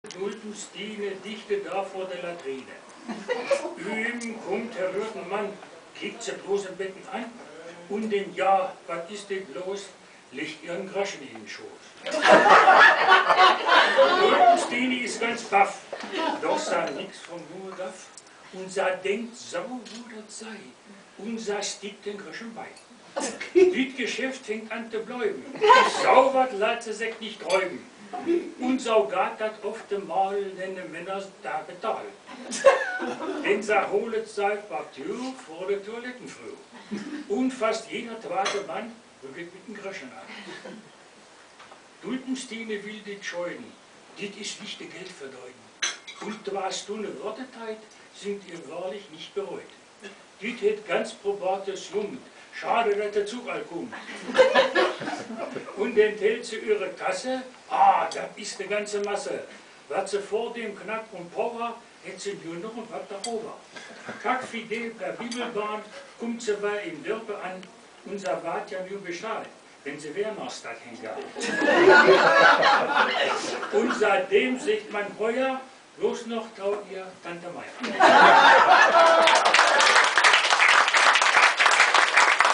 Kleine Geschichte auf Plattdeutsch
Aufgenommen am Tag der offenen Tür im Heimatmuseum Holle bei Hildesheim.